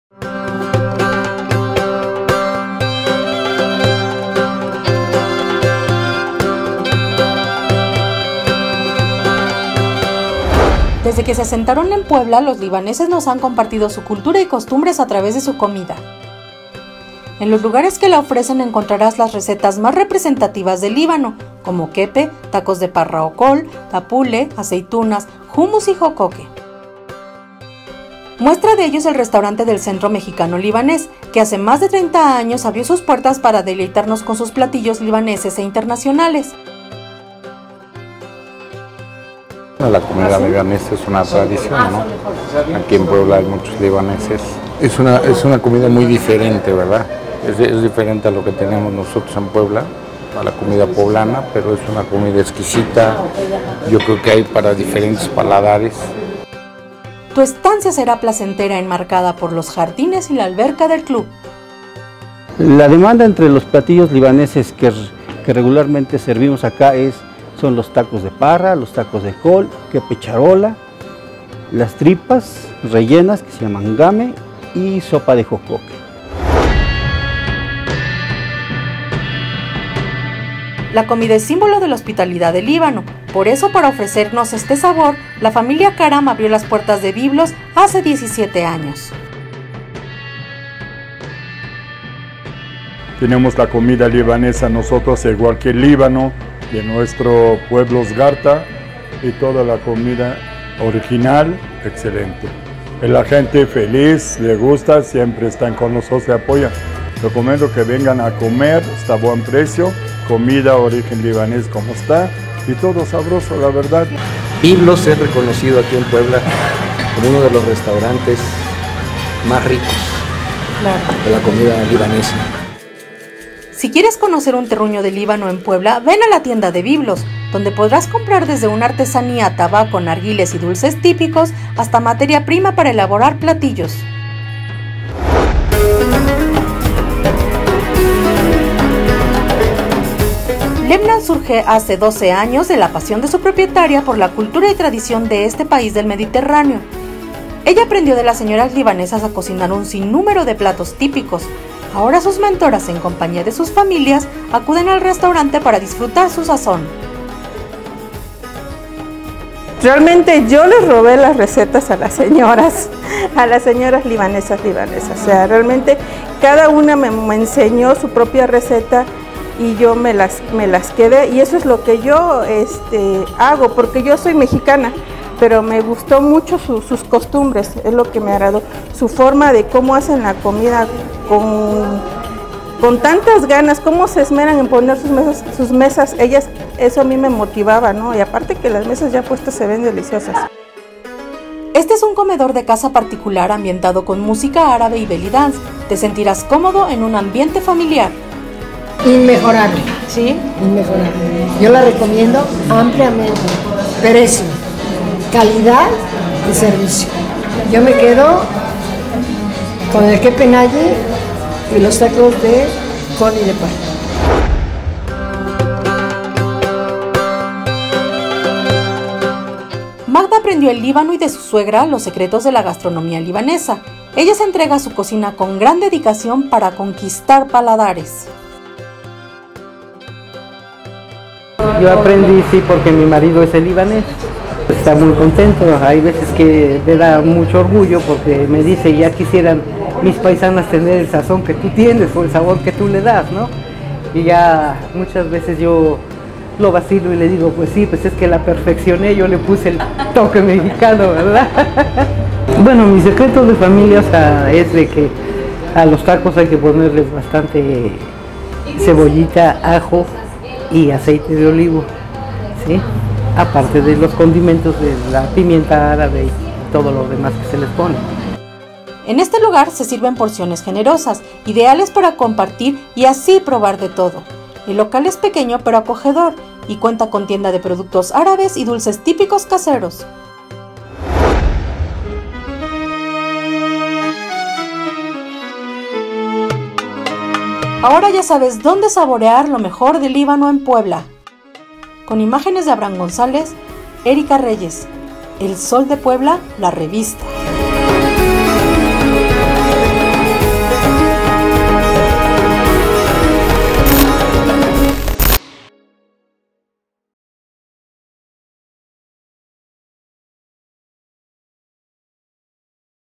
Aquí podrás comer comida libanesa en Puebla
Degusta un sabor distinto y deleitate con el sazón del Libano